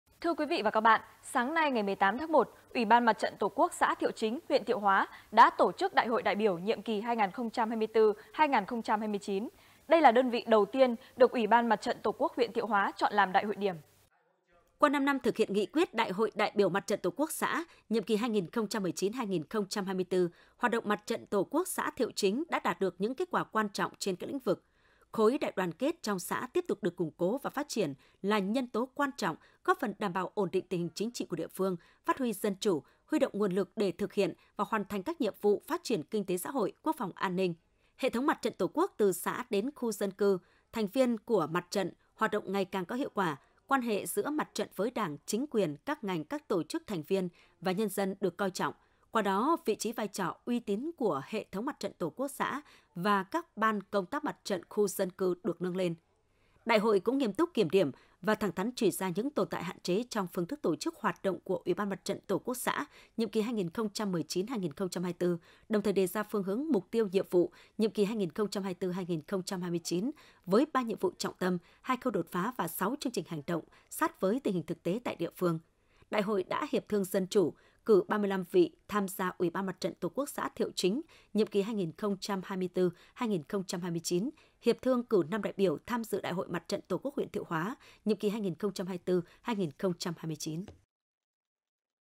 Đại hội đại biểu nhiệm kỳ 2024 2029 Ủy ban MTTQ xã Thiệu Chính
Sáng ngày 18/1, Ủy ban MTTQ xã Thiệu Chính, huyện Thiệu Hoá đã tổ chức Đại hội đại biểu nhiệm kỳ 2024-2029. Đây là đơn vị đầu tiên được Ủy ban MTTQ huyện Thiệu Hóa chọn làm đại hội điểm.